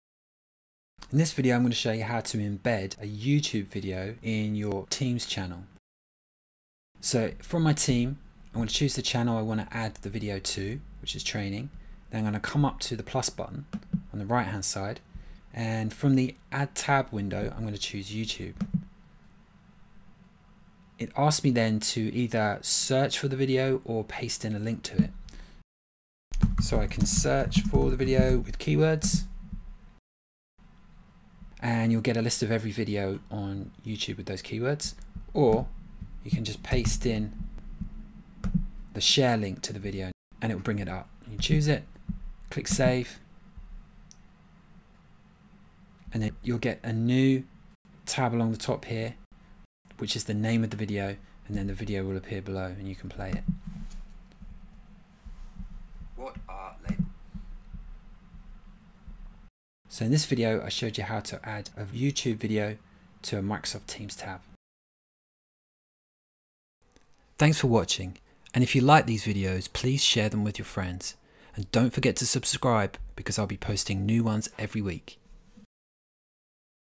How to Add a YouTube Video to Microsoft Teams
In this video I’ll show you how to add a YouTube video to Microsoft Teams.  I’ll add a new tab to an existing Teams channel and then embed the YouTube video in it.